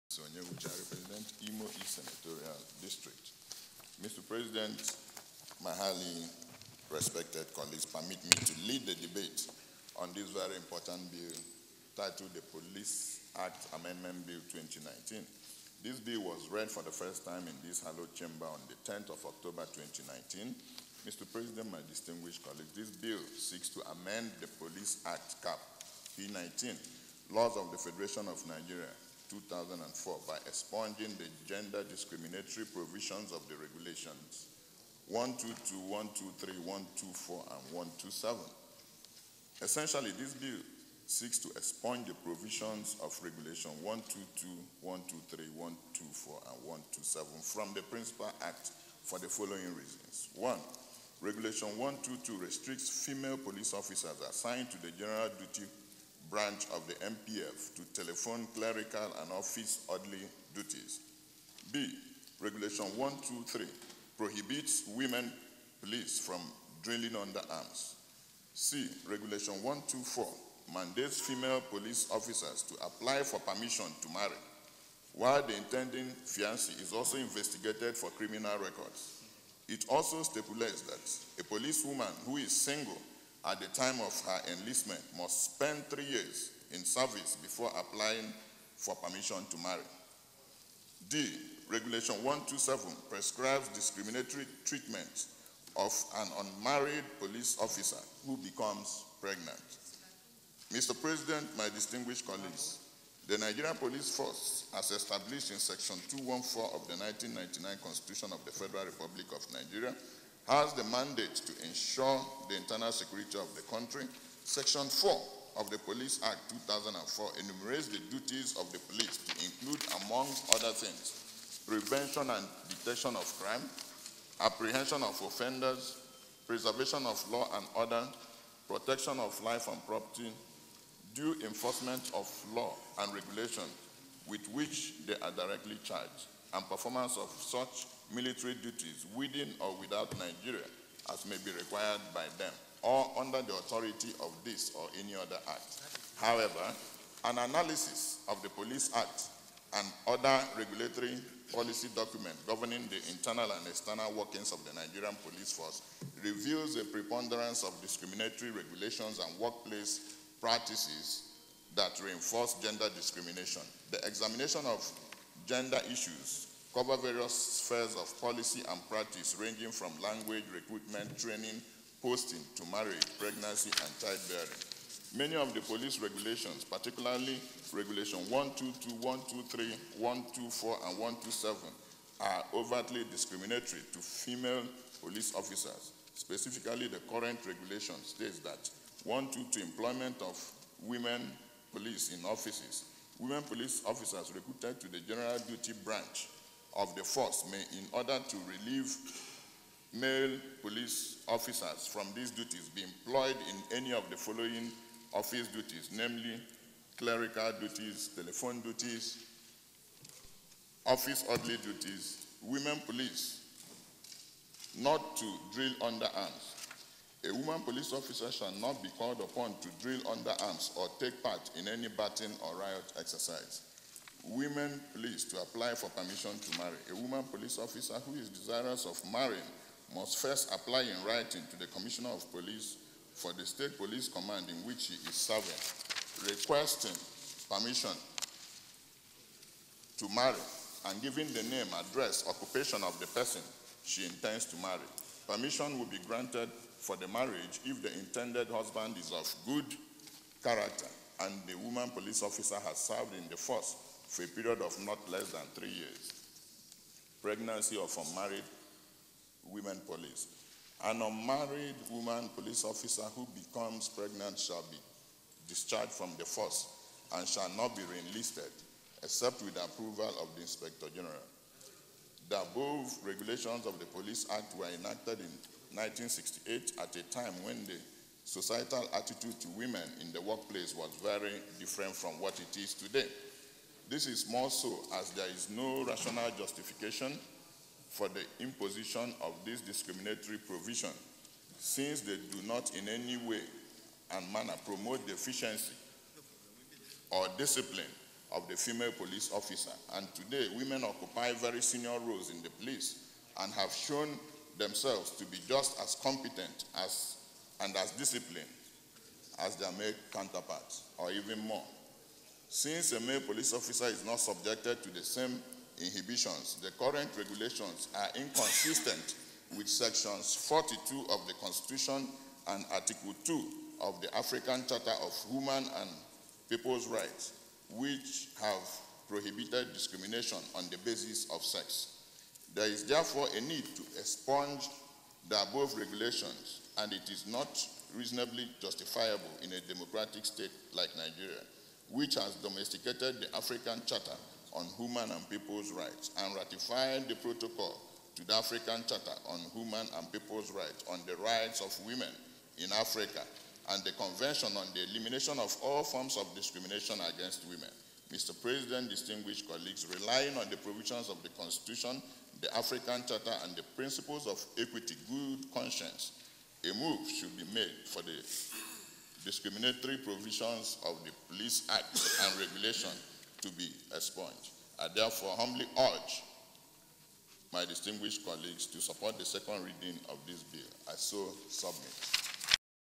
Senate Moves To End All Forms Of Discrimination Against Female Police Officers
A bill seeking to provide equal rights for female police officers as their male counterparts has passed second reading on the floor of the senate. The bill which seeks to amend the Police Act CAP P19 LFN 2004 and expunge the gender discriminatory provisions of regulations 122, 123, 124 and 127 for other related matters, 2019 (SB.